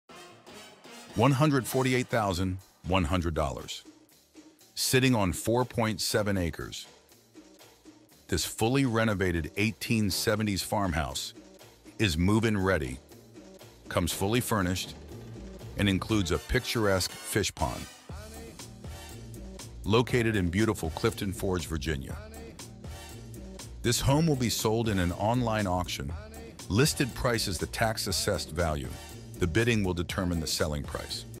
Win This Dream Farmhouse! (Not Haunted 👻) $148K Starting Bid | 4.7 Acres in Virginia
Win This Dream Farmhouse! $148K Starting Bid | 4 7 Acres & Furnished in Virginia

6400 Longdale Furnace Rd Clifton Forge VA 24422

This home is being sold in an online auction. Listed price is the tax assessed value and not the selling/asking price. The bidding will determine the selling price. Discover the charm of a fully renovated 1870s farmhouse, nestled on 4.7 acres with a picturesque fish pond. This turnkey, move-in-ready home comes fully furnished. Make this your easiest move ever and open the door and lay in your new king size bed or make this your new rental property!

🎧 100% royalty, Free Music for videos with free downloads by David Cutter